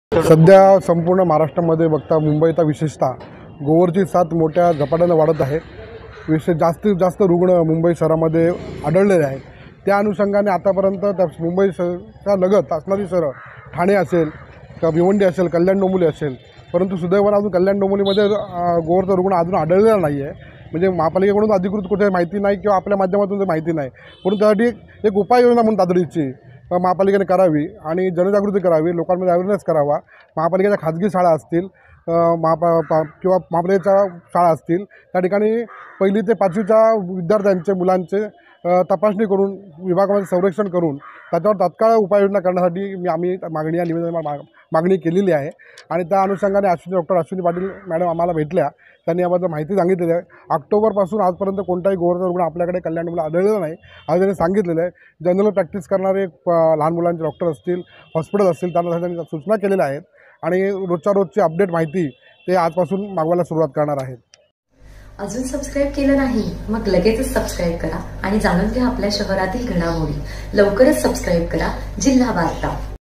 गोवर साथीवर तातडीने उपाययोजना करा,पालांडे यांच्या शिष्टमंडळाने यासंदर्भात महापालिकेत दिले निवेदन....
गोवर रुग्णांची संख्या वाढत असल्याने त्यावर तातडीने उपाययोजना करा, अशी मागणी उद्धव ठाकरे शिवसेना पक्षाचे उपजिल्हा प्रमुख हर्षवर्धन पालांडे यांनी कल्याण-डोंबिवली महापालिका आयुक्त डॉ. भाऊसाहेब दांगडे यांच्याकडे केली आहे. पालांडे यांच्या शिष्टमंडळाने बुधवारी आयुक्तांची भेट घेऊन त्यांना यासंदर्भात निवेदन दिले.